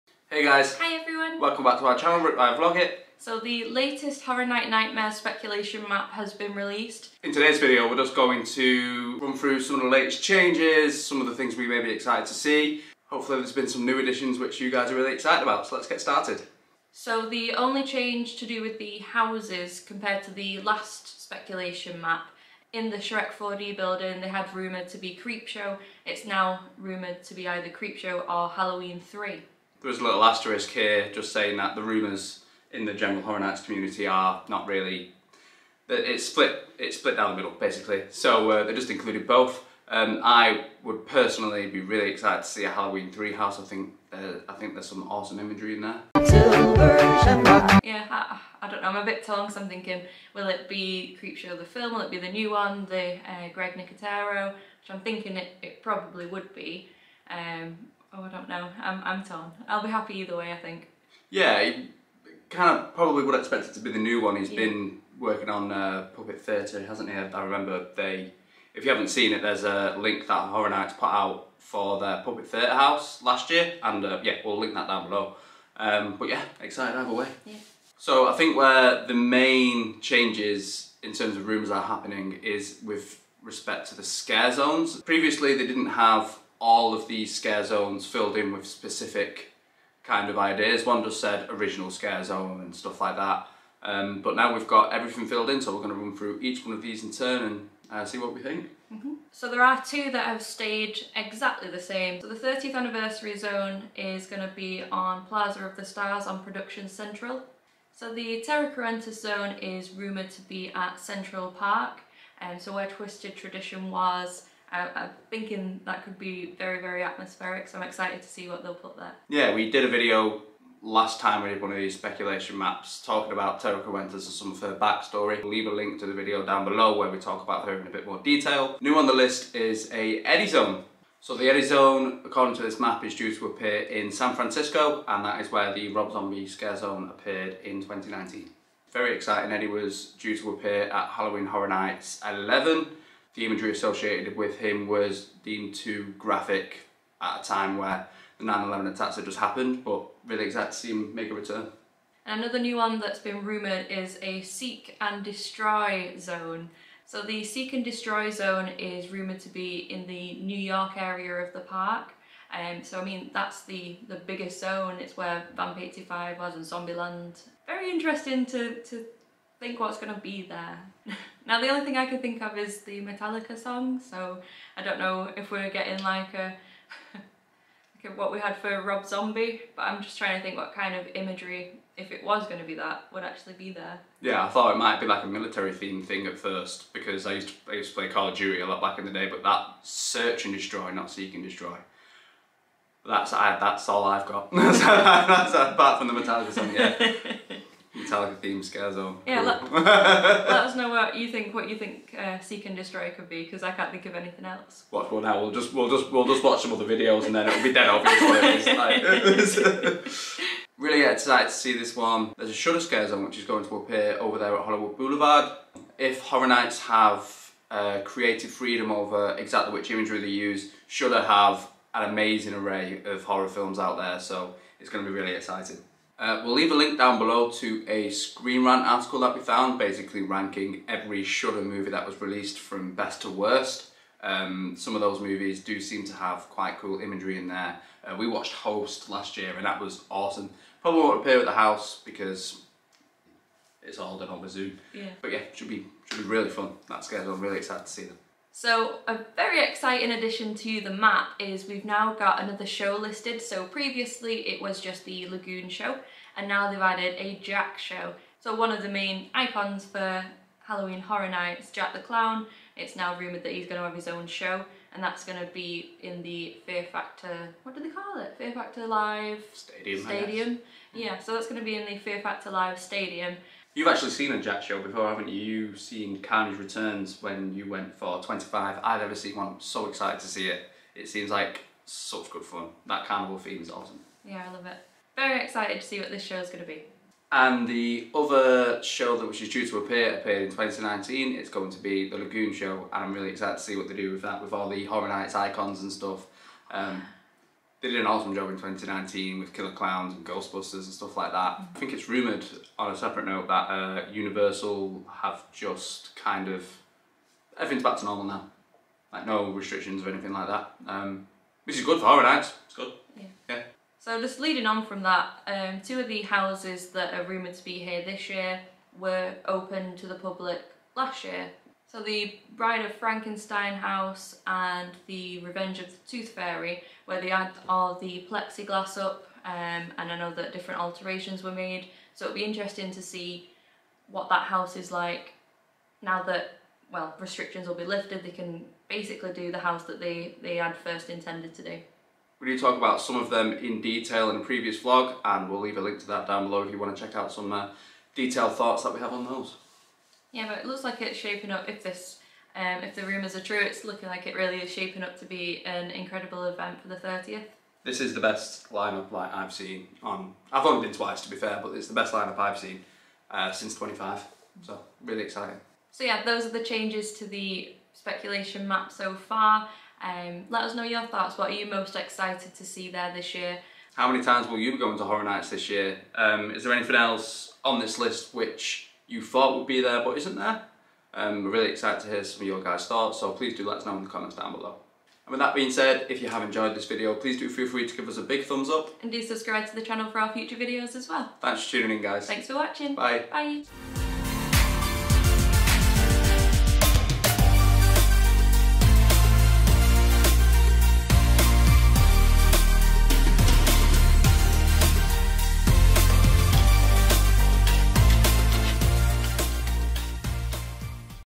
HHN 2021 Rumours | Horror Night Nightmares Speculation Map V2
Hi Everyone!

A new speculation map just dropped from Horror Night Nightmares! A lot of changes in this compared to the previous version!

We discuss the new additions to the scarezones, some of the newly disputed ideas from the community about what will be in the Shrek 4D building, and more!

If the Shudder scarezone comes to fruition, we are very excited to see this.

We hope you enjoy this weeks video!

If you’d like to keep up to date with Horror Night Nightmares their Twitter is

- We discuss houses in more detail

Laura & Dane x